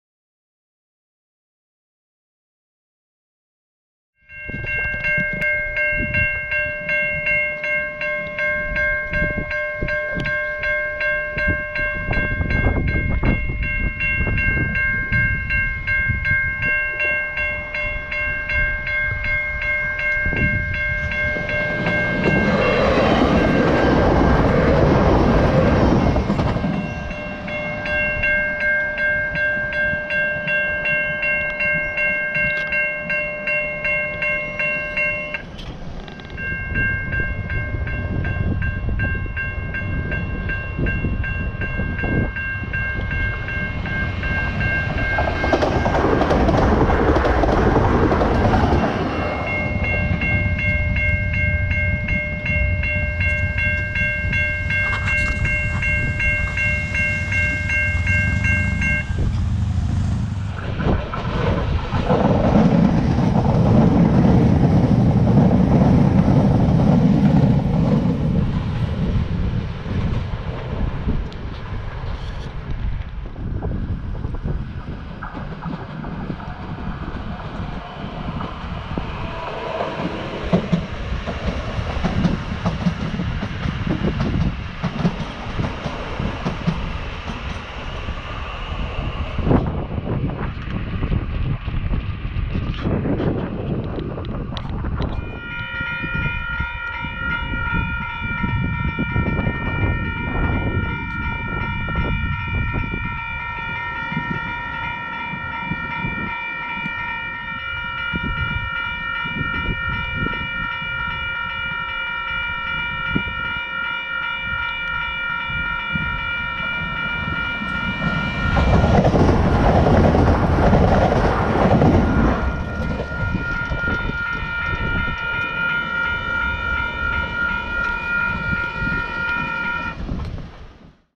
Trains at Sunnyvale & Railside Ave
So i recorded at 3 spots all across henderson
-Railside ave
-Sunnyvale
-Sherrybrooke Place

And i was trying some new angles on my camera